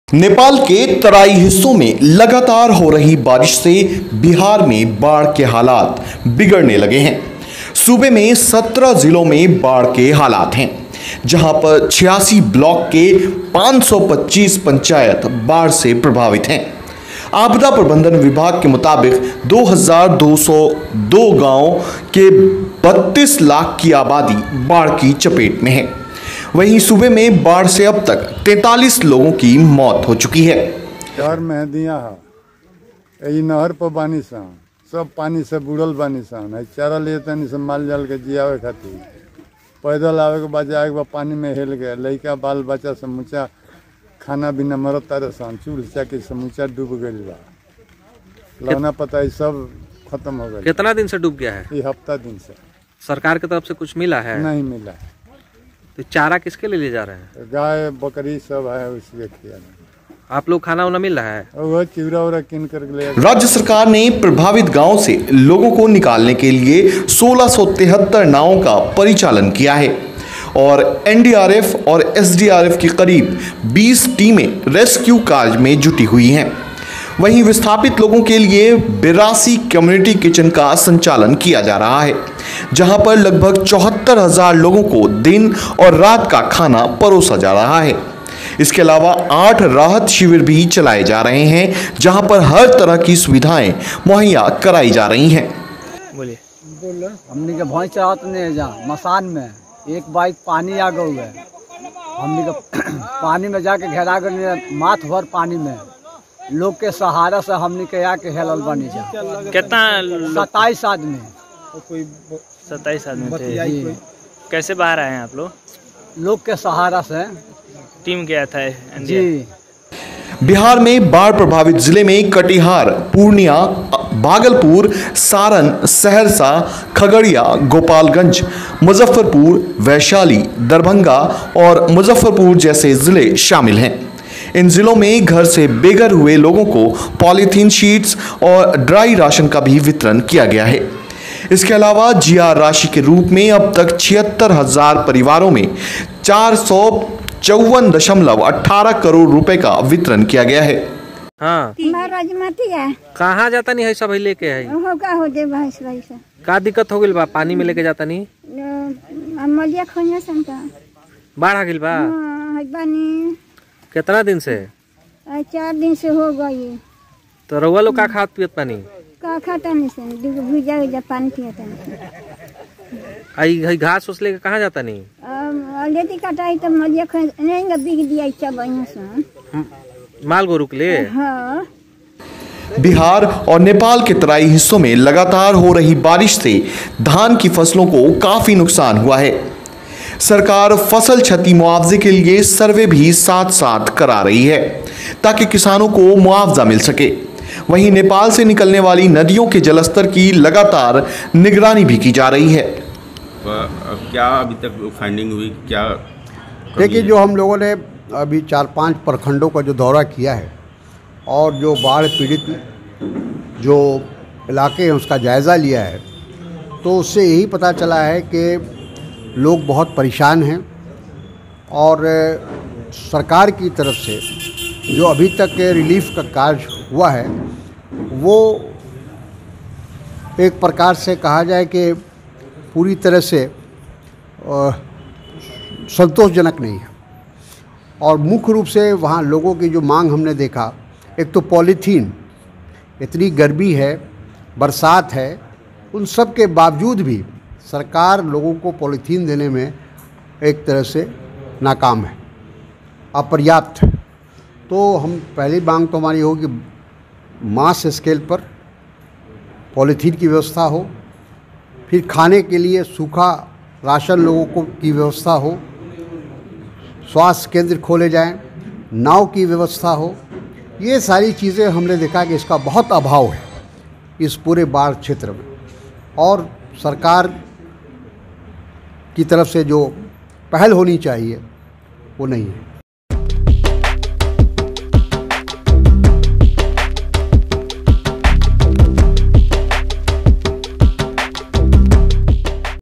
Bihar Flood Updates : नेपाल से पानी छोड़ने पर बिहार के 17 जिलो में तबाही, बाढ़ से 30 लाख लोग बेघर
नेपाल के तराई हिस्सों में लगातार हो रही बारिश से बिहार में बाढ़ के हालात बिगड़ने लगे हैं। सूबे में 17 जिलों में बाढ़ के हालात हैं। जहां पर 86 ब्लॉक के 525 पंचायत बाढ़ प्रभावित हैं। आपदा प्रबंधन विभाग के मुताबिक 2202 गांवों के 32 लाख आबादी बाढ़ की चपेट में है। वहीं सूबे में बाढ़ से अब तक 43 लोगों की मौत की हुई है।
राज्य सरकार ने प्रभावित गांवों से लोगों को निकालने के लिए 1673 नावों का परिचालन किया है और एनडीआरएफ और एसडीआरएफ की करीब 20 टीमें रेस्कूय कार्य में जुटी हुई हैं। वहीं विस्थापित लोगों के लिए 82 कम्यूनिटी किचन का संचालन किया जा रहा है। जहां पर लगभग 74 हजार लोगों को दिन और रात का खाना परोसा जा रहा है। इसके अलावा 8 राहत शिविर भी चलाए जा रहे हैं। जहां पर हर तरह की सुविधाएं मुहैया कराई जा रही हैं।

बिहार में बाढ़ प्रभावित जिले में कटिहार, पूर्णिया, भागलपुर, सारण, सहरसा, खगड़िया, गोपालगंज, मुजफ्फरपुर, वैशाली, दरभंगा, मुजफ्फरपुर जैसे जिले शामिल हैं। इन जिलों में घर से बेघर हुए लोगों पॉलिथिन शीटस और ड्राई राशन का भी वितरण किया गया है। इसके अलावा जीआर राशि के रुप में अब तक 76 हजार परिवारों में 454.18 करोड़ रुपए का वितरण किया गया है।

बिहार और नेपाल के तराई हिस्सों में लगातार हो रही बारिश से धान की फसलों को काफी नुकसान हुआ है। सरकार फसल क्षति मुआवजा के लिए सर्वे भी साथ साथ करा रही है। ताकि किसानों को मुवाअजा मिल सके । वहीं नेपाल से निकलने वाली नदियों के जलस्तर की लगातार निगरानी भी की जारी है।